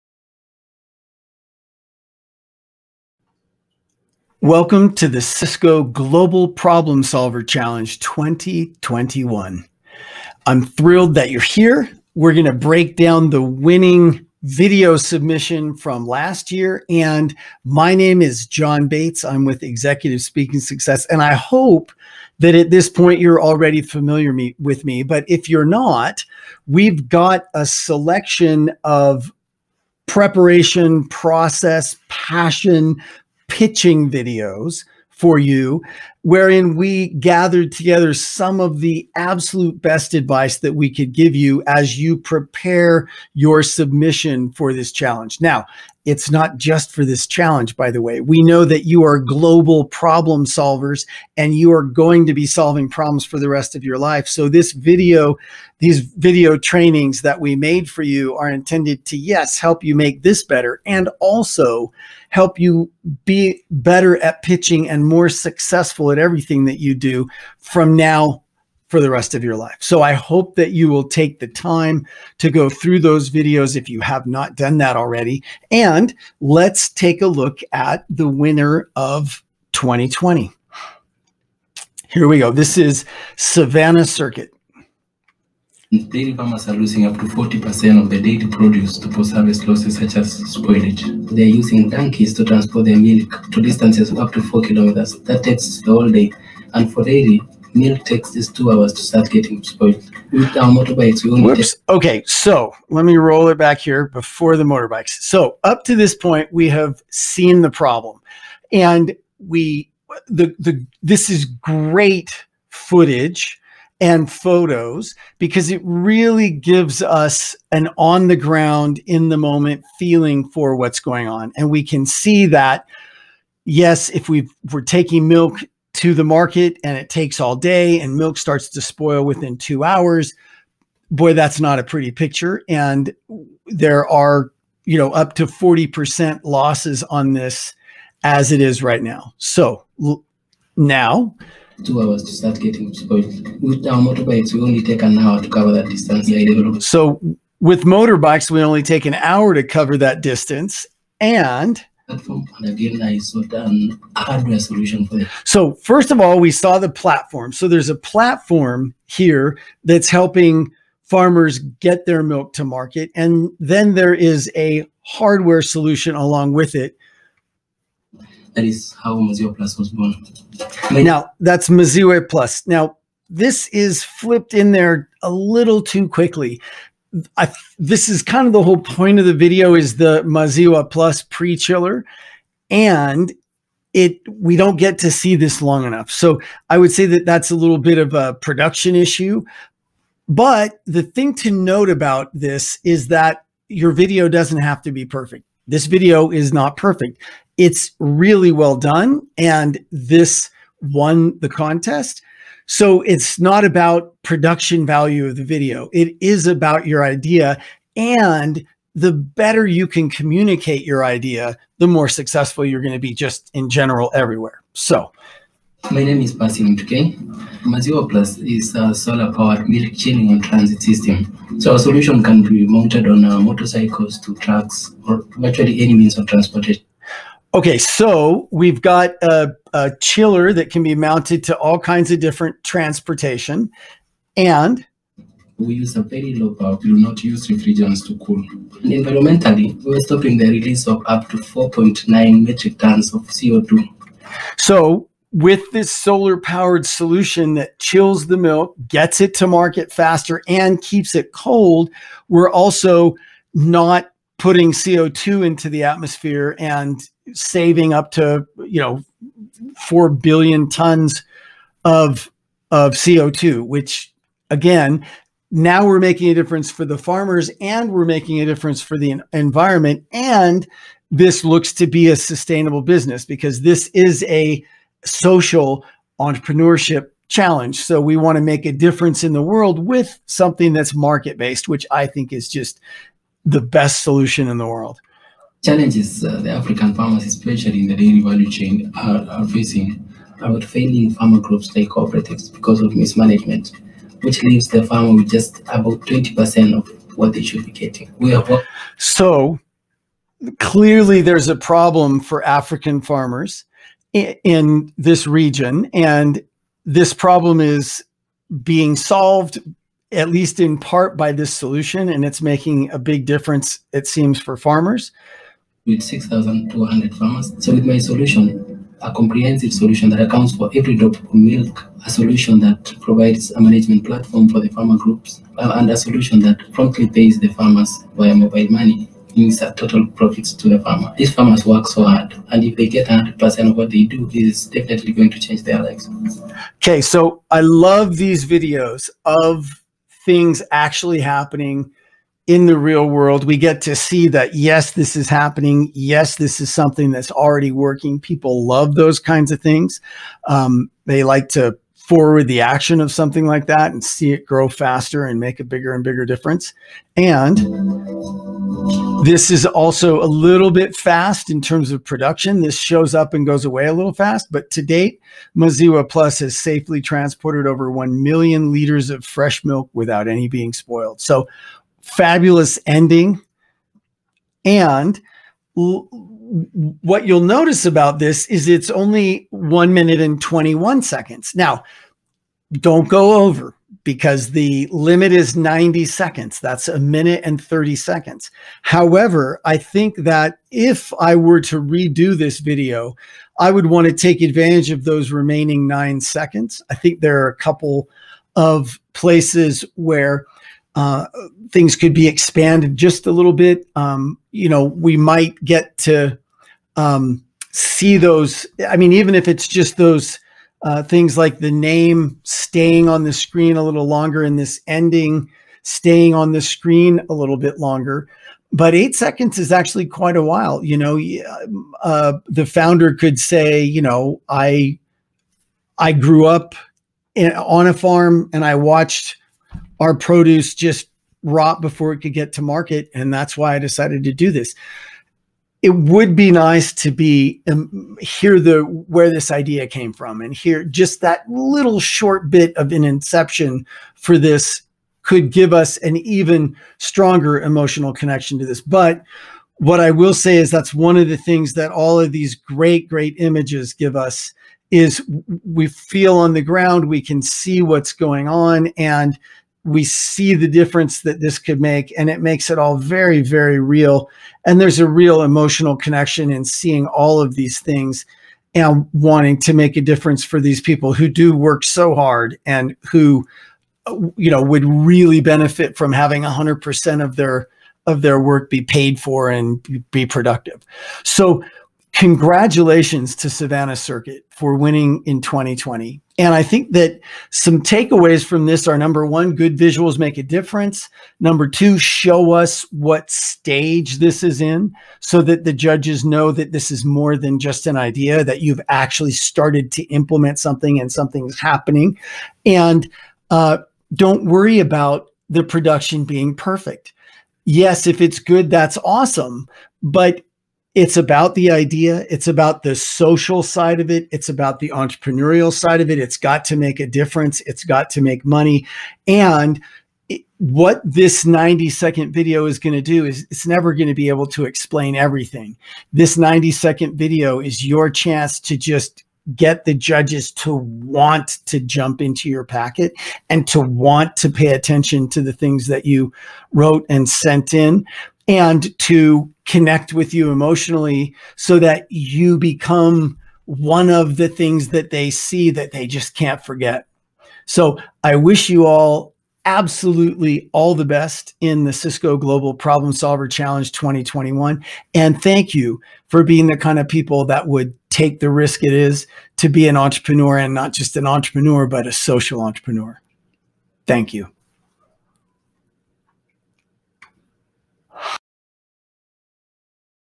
Pitch Breakdown of Cisco Global Problem Solver Challenge 2020 Winner: Savanna Circuit
Expert pitch coach, John Bates, reviews the 2020 winning team's video and highlights why this pitch was successful.

A solar chilling transit system with dairy management system for dairy businesses/ cooperatives in sub-Saharan Africa to cut post harvest losses.

Refrigeration is key in agricultural value chains, to prolong shelf life and preserve quality of produce. Emerging markets have smallholders as main producers who lack proper refrigeration capacities resulting in 30% post-harvest losses.

Savanna Circuit manufactures & distributes last mile non-refrigerant solar powered milk chillers placeable on any means of transportation from motorbikes to trucks to cut down on milk spoilage for maximized profits to dairy producers/processors with an AI-enabled ICT solution.